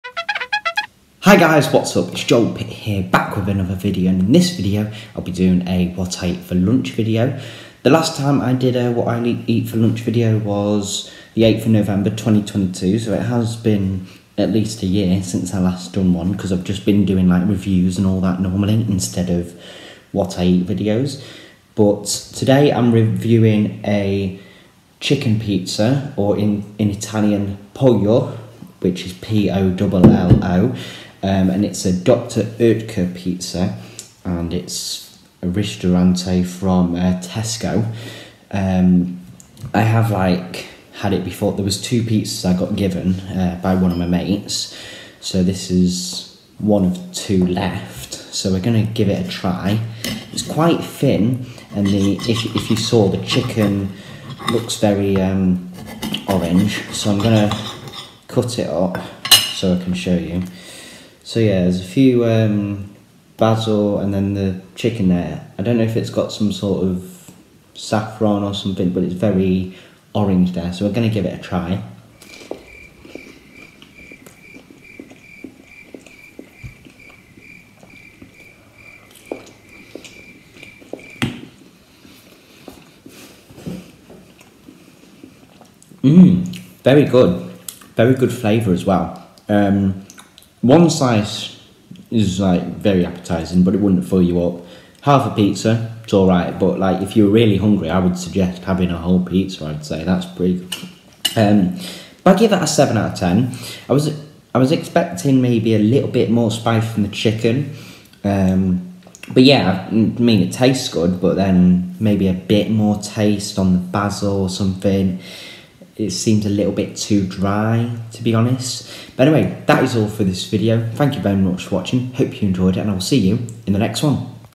102nd video of what I eat for lunch at my house.
Hello everyone and welcome to another video, in this video I will be doing a what I eat for lunch video. Today with me I’ve got a Pizza Pollo from Dr Oketer. It is basically a chicken pizza I think from Tesco. The last time I did a what I eat for lunch video was on the 8th November 2022.